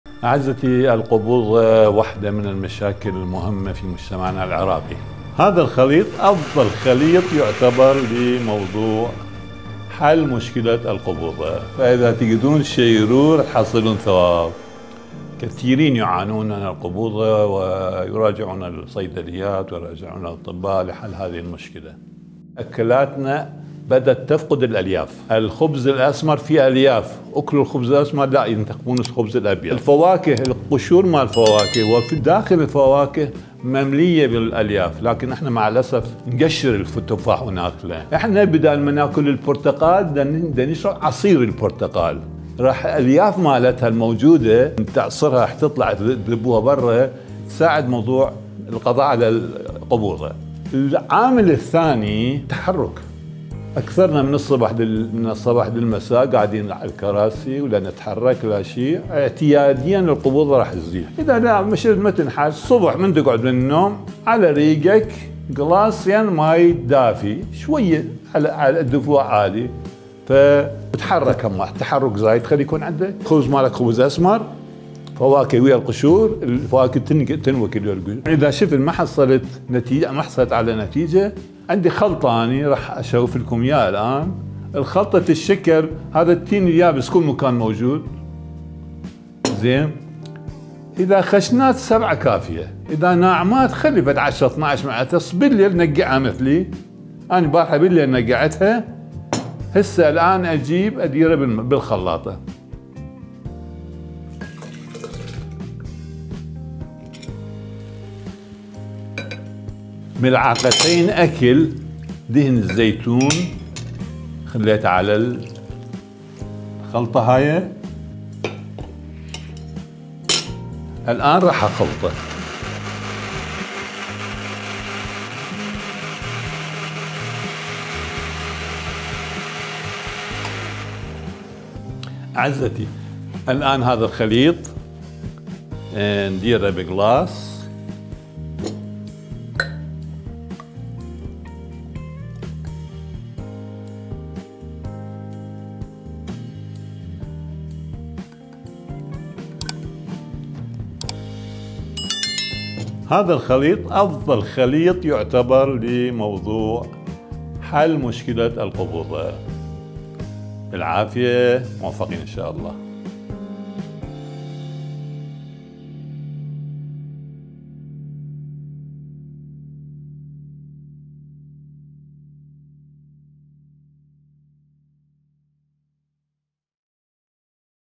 علاج الإمساك في المنزل: بدون أدوية أو مراجعات !
خلطة سحرية من الدكتور محمد الكرماني لعلاج الإمساك،علاج الإمساك في المنزل بدون أدوية أو مراجعات ! علاج الإمساك بطريقة سهلة وبسيطة، تستطيع فعلها  في المنزل بدون أي صعوبة أو تكلفة وتجنبك الأدوية والمراجعات. كيفية علاج الإمساك بسرعة فائقة.

🔰أفضل طريقة صحية وسليمة للتخلص من السمنة والنحافة تحت إشراف خبير التغذية الدكتور محمد الكرماني.